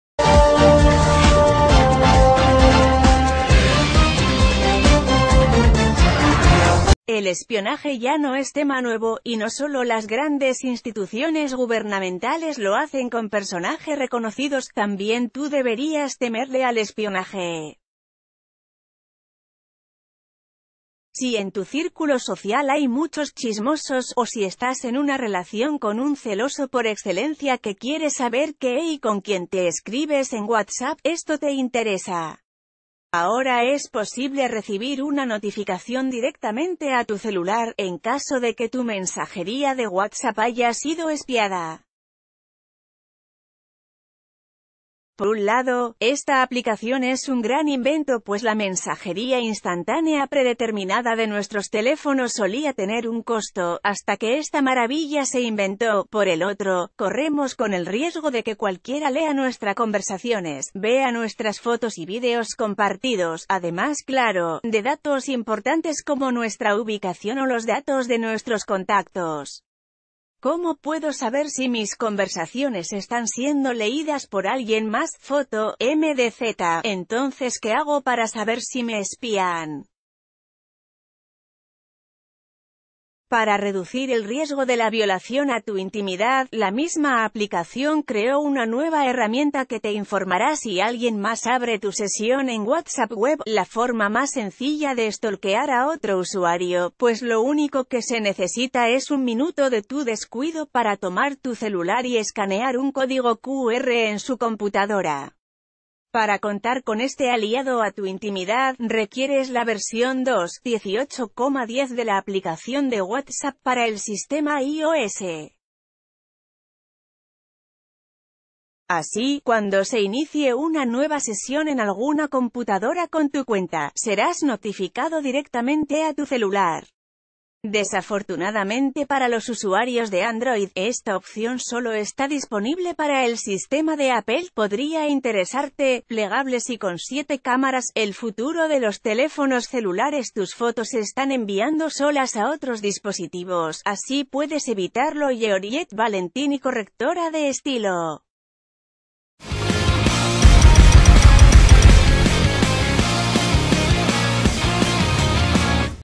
¿Cómo recibir una notificación a tu celular si alguien espía tu WhatsApp?
El espionaje ya no es tema nuevo, y no sólo las grandes instituciones gubernamentales lo hacen con personajes reconocidos. También tú deberías temerle al espionaje. Si en tu círculo social hay muchos chismosos, o si estás en una relación con un celoso por excelencia que quiere saber qué y con quién ...